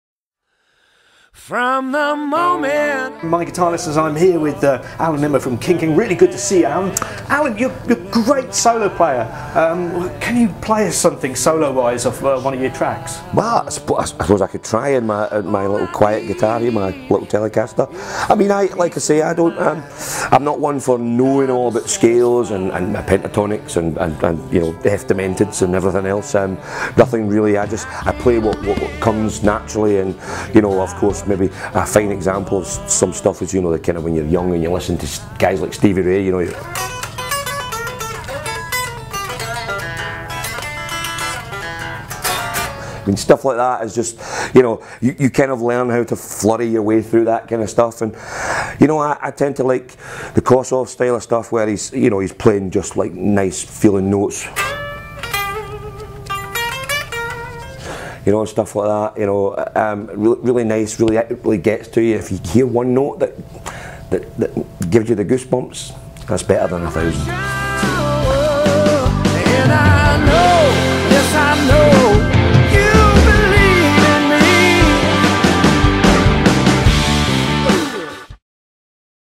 Alan Nimmo From The Band King King Plays A lead solo guitar Lesson
King Kings amazing guitarist shows us some SRV and Free licks that he picked up in his youth, and advises all guitar students learning to play the guitar to get to grips with this type of playing in their lessons. so keep logging back in.